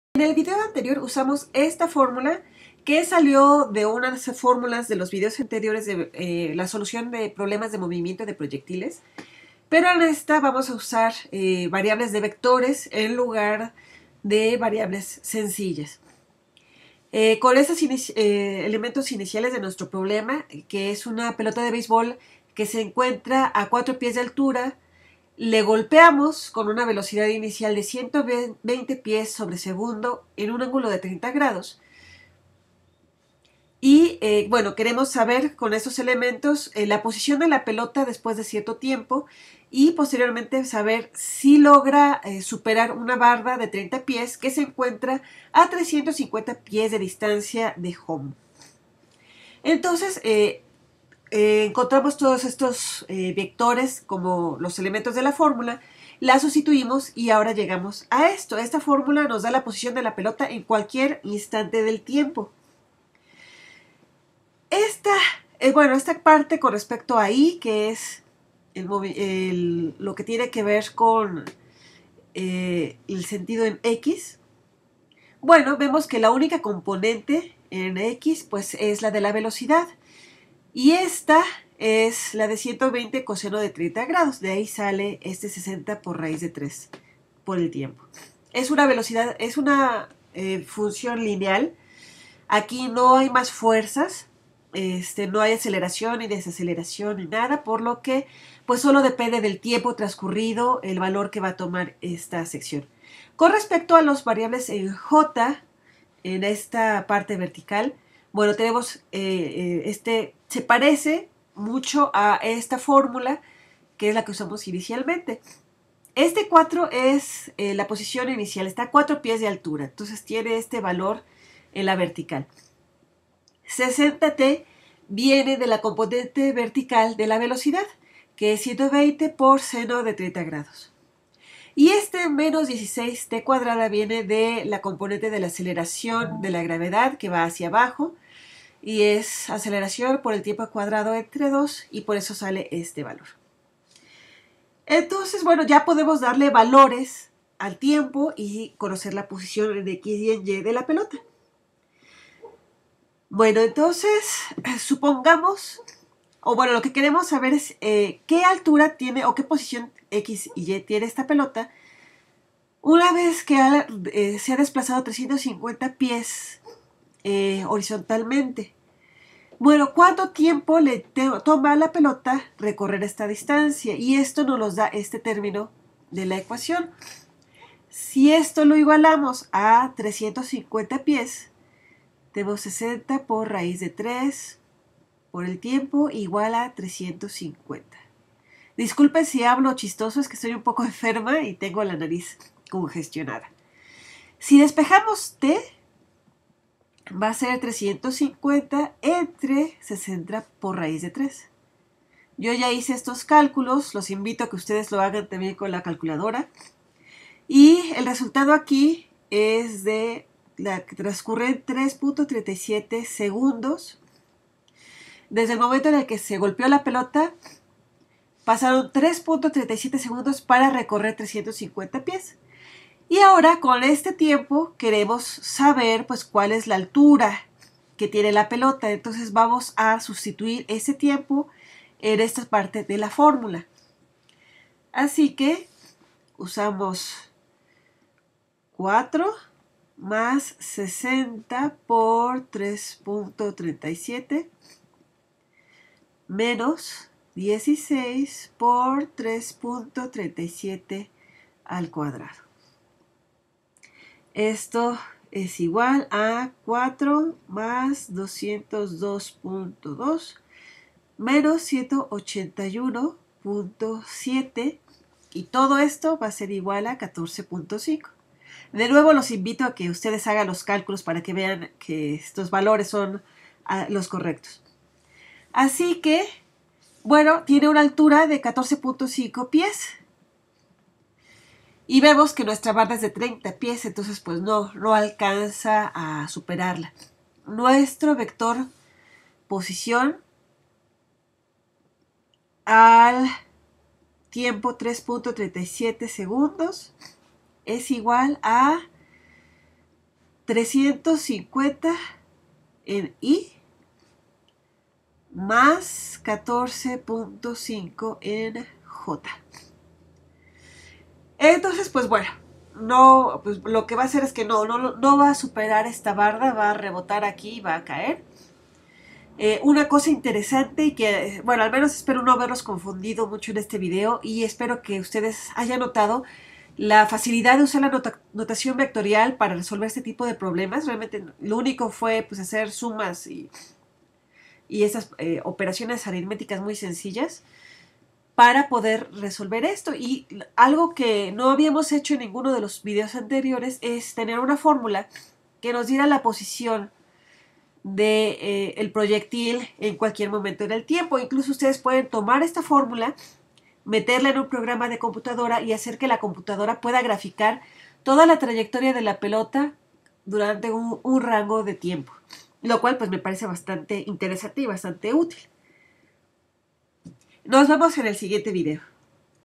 Movimiento de proyectiles usando notacion vectorial 2p
Usando notación vectorial para resolver un problema de movimiento de proyectiles. Función de posición del proyectil con respecto al tiempo.

Traducido para Khan Academy por Lourdes Cahuich